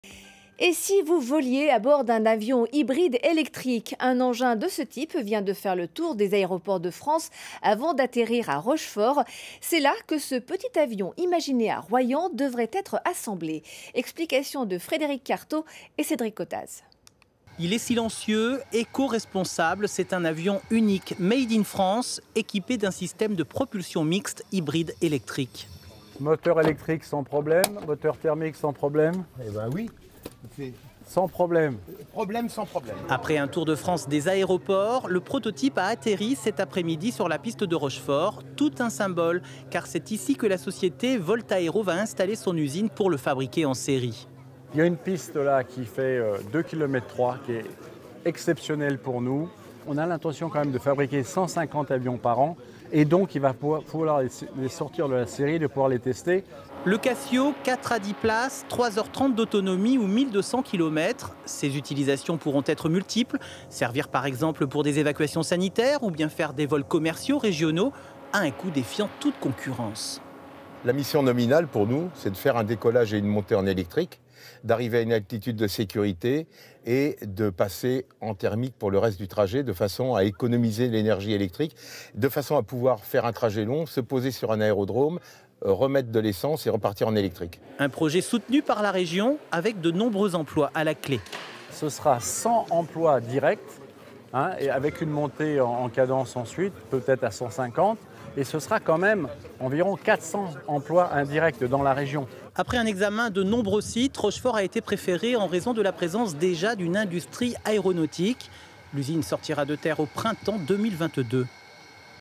Voltaéro installe son usine de fabrication d'avions hybrides à Rochefort en Charente-Maritime
On vous l'a présenté il y a quelques mois : un avion mixte hybride électrique a été mis au point à Royan par des ingénieurs de l'aérospatiale. Il était présenté ce dimanche 11 juillet à Rochefort, car c'est là-bas que la société Voltaéro, propriétaire des brevets, vient de décider d'installer son usine d'assemblage. Entre 100 et 150 emplois à la clef à l'horizon fin 2022. Reportage de Frédéric Cartaud, Cédric Cottaz et Thierry Cormerais
Intervenants :
- Jean Botti, PDG de la société VoltAéro
- Didier Efteyne, directeur technique, pilote d'essai